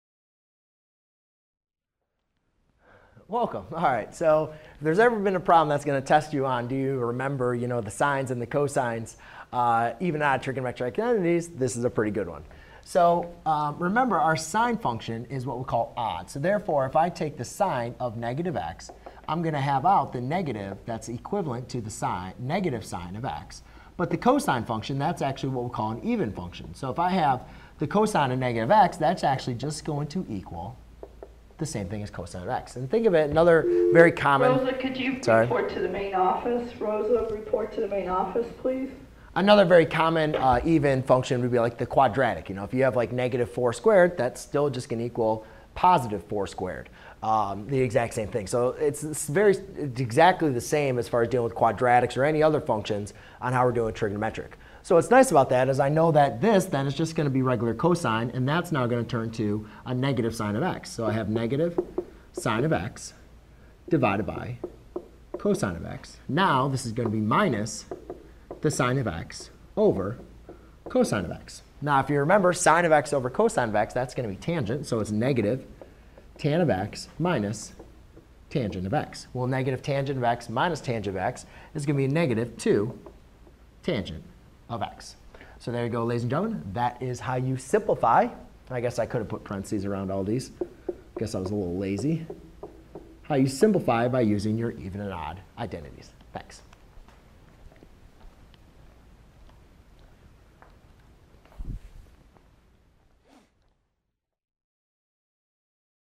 Simplify by finding the sum of rational expressions using even odd identities
👉 Learn how to simplify rational identities involving addition and subtraction. To simplify rational identities involving addition and subtraction, first, we find the LCM of the denominators which most time is the product of the terms in the denominators.
After we have found the LCM of the denominators, we next, multiply the numerator and the denominator of each rational identity with a factor that makes the denominator equal to the LCM obtained. This leaves all the rational identities with the same denominators, we can then add/subtract the numerator.

Common trigonometric identities like the quotient, reciprocal, even, odd, co-function, and Pythagoras trigonometric identities can be applied to further reduce the original trigonometric identity.